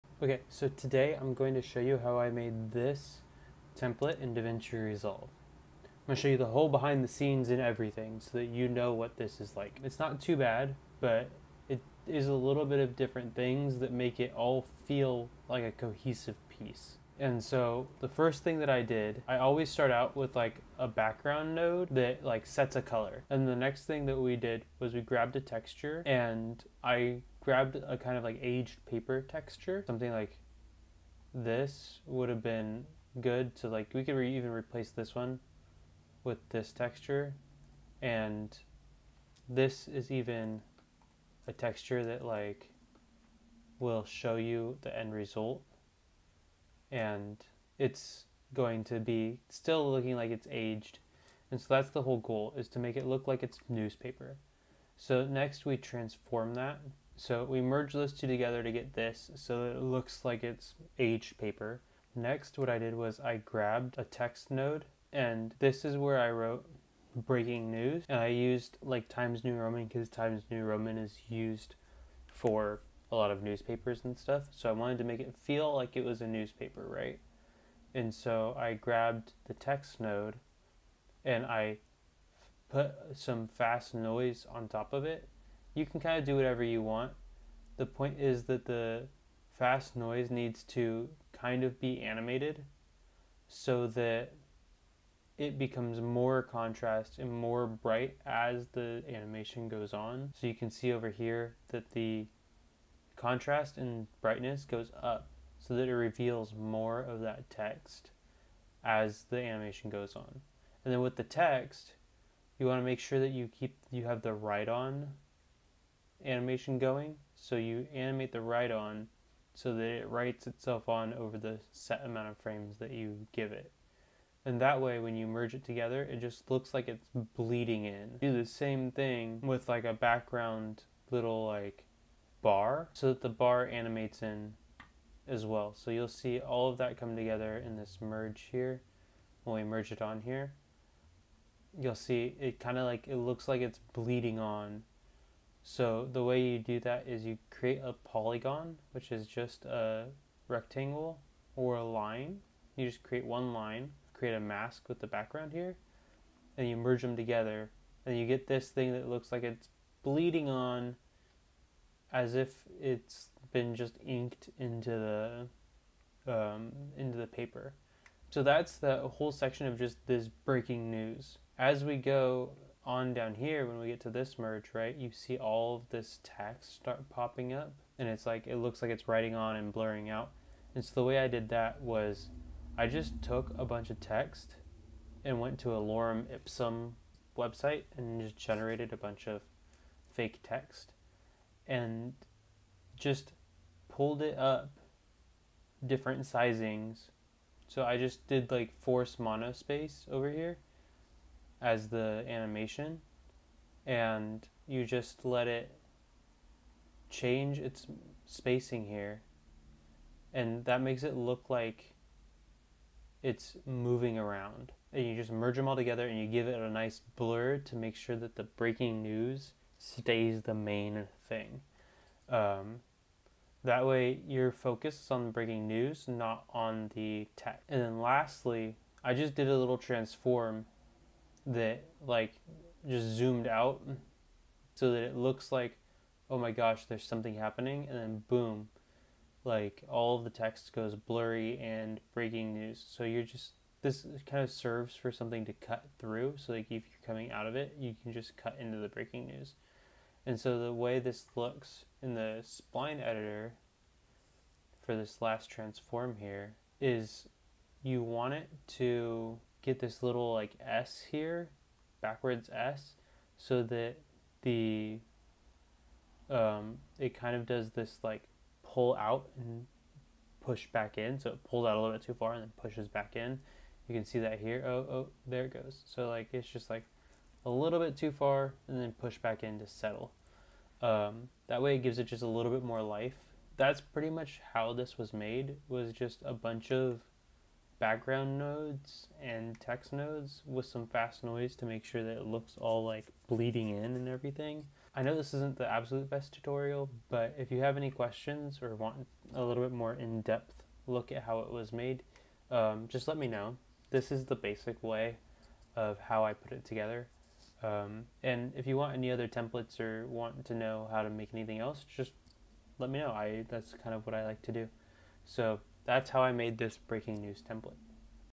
Breaking News Motion Graphic Template - DaVinci Resolve Fusion
How I made a template for an aged breaking news motion graphic.